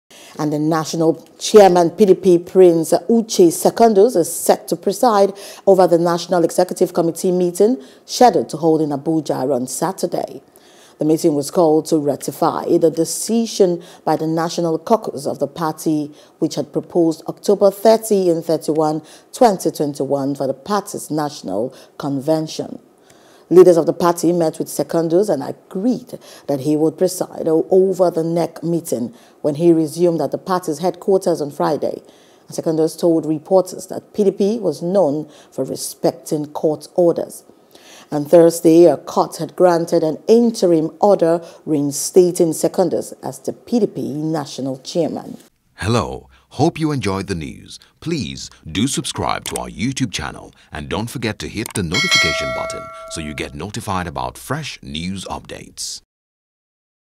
Secondus To Preside Over NEC, Convention Tops Agenda | NEWS
The National Chairman PDP, Prince Uche Secondus, is set to preside over the National Executive Committee meeting scheduled to hold in Abuja on Saturday.